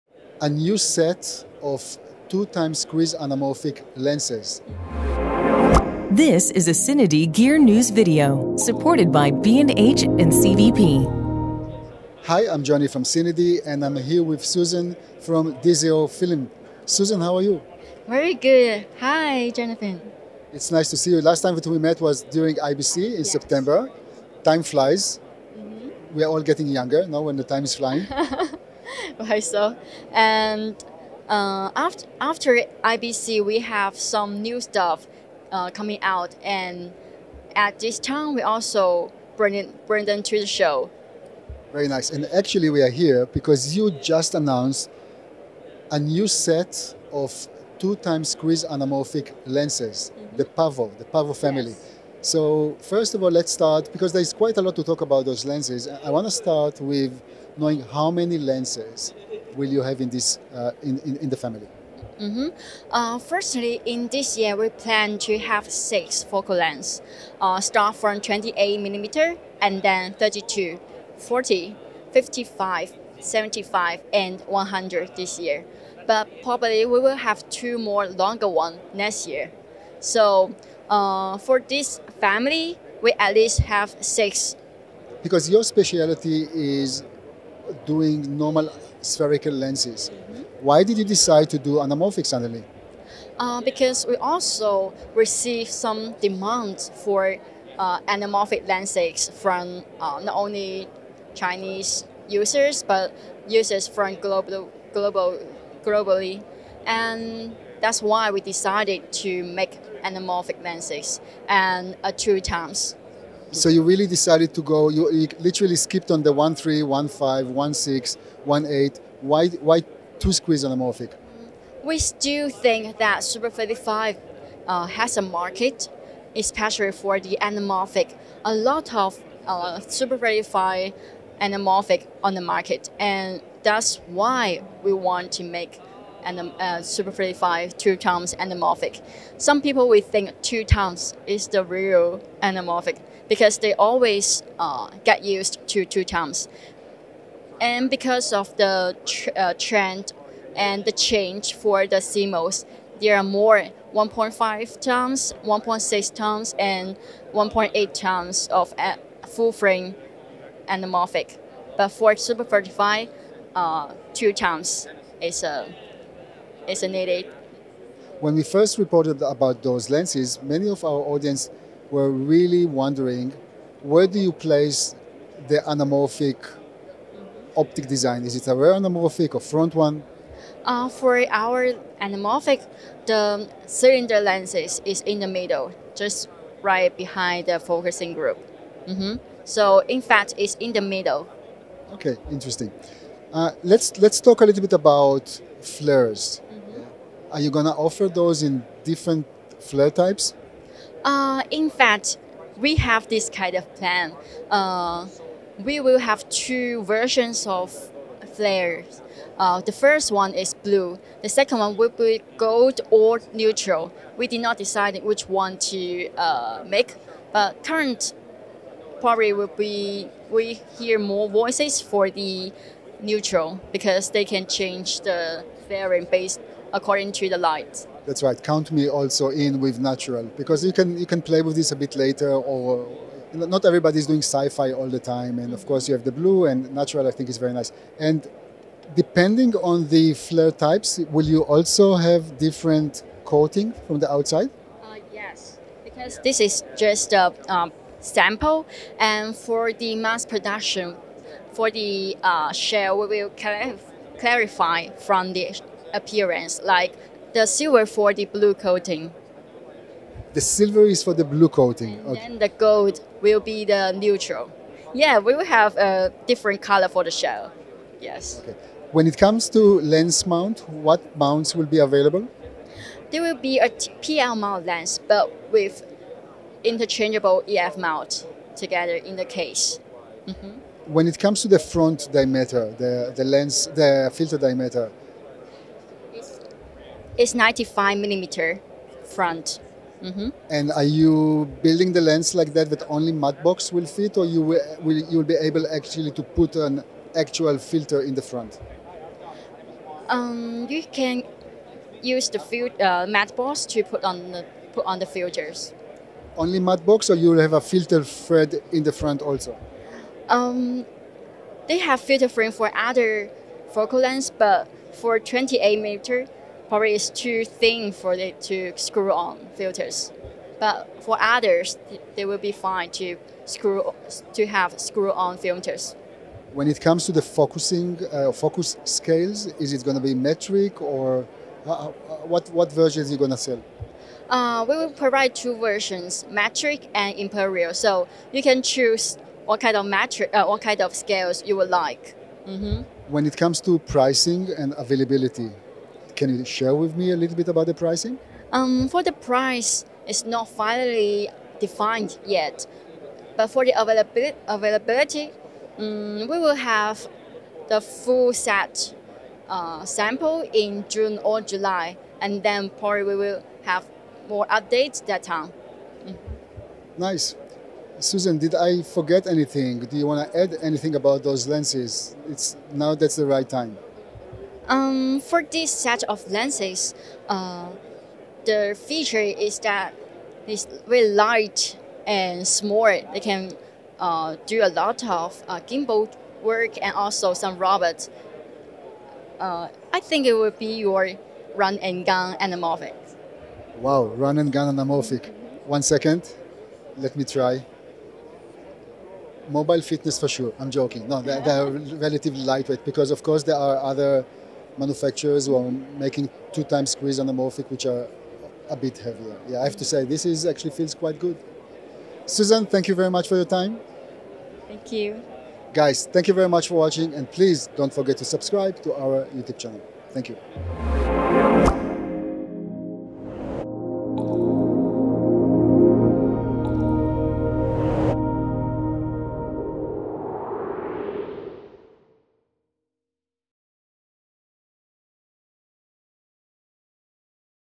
DZOFILM PAVO 2X Anamorphic Lens Series – First Look
Right before NAB 2023, Chinese lens manufacturer DZOFILM teased its first-ever anamorphic lens series, called PAVO. Their new adventure starts in full swing with the introduction of a 6-piece lens set featuring a cinematic 2X squeeze factor.